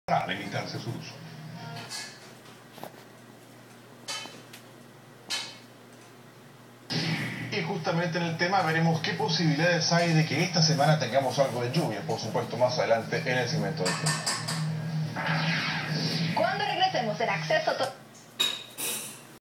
full spider on traffic cam lol?
i was watching the news and on the background was a big spidah just chillin